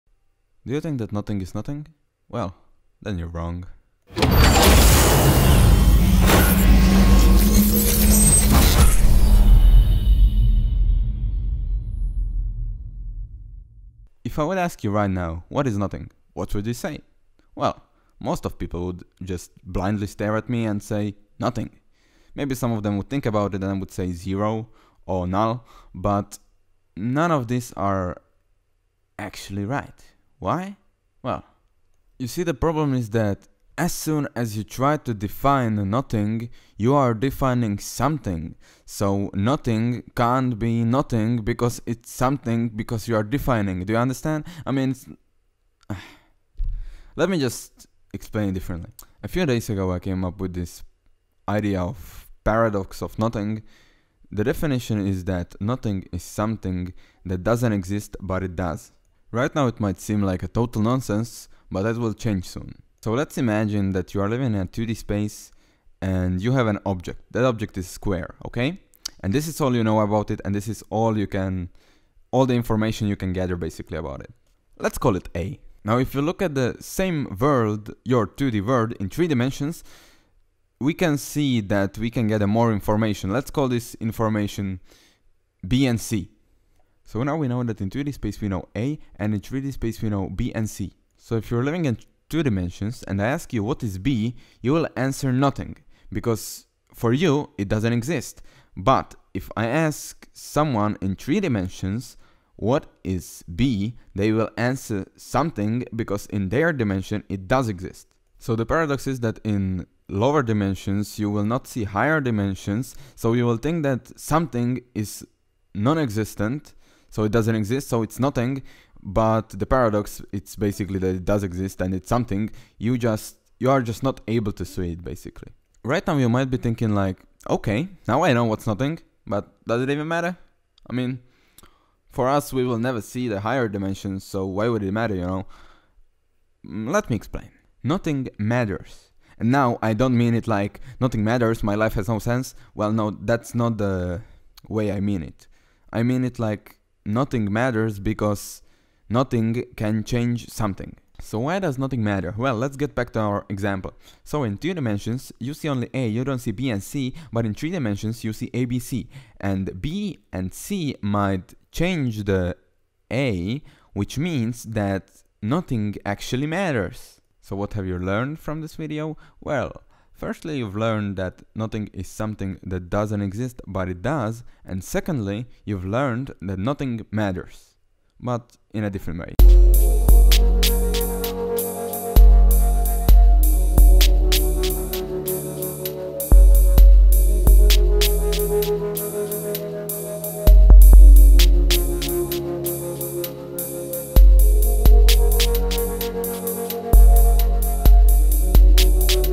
The Paradox of Nothing
A few days ago I came up with an idea to describe nothing, I tried to explain nothing as simply as possible in this video. I called my idea "The Paradox of Nothing". Oh, and I also explained why nothing matters. Everything is explained very simply without the need for any other knowledge.